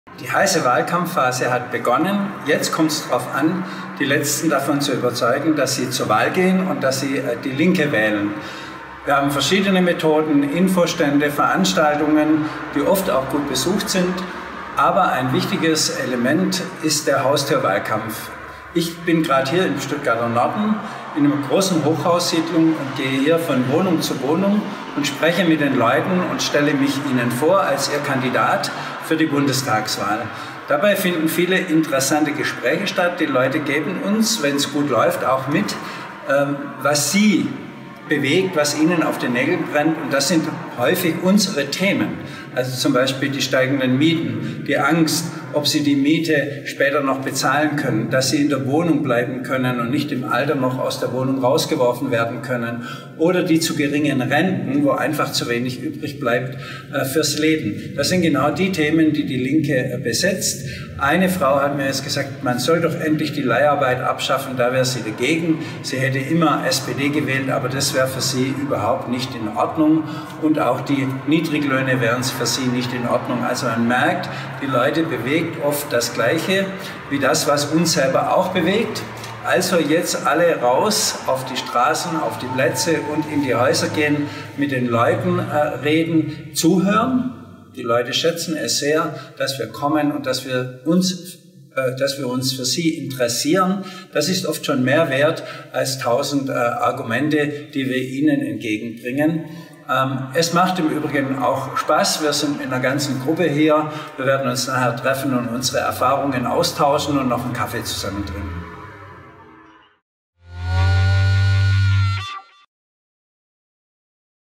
Los geht's: Haustürwahlkampf
„Zuhören, die Leute schätzen es sehr, dass wir kommen und dass wir uns für sie interessieren“, so die Erfahrung unseres Parteivorsitzenden Bernd Riexinger vom Haustürwahlkampf der LINKEN in Baden-Württemberg. Also – auf in die heiße Wahlkampfphase. Ob von Tür zu Tür oder bei anderen Begegnungen: Hören und sagen, was ist! Mach mit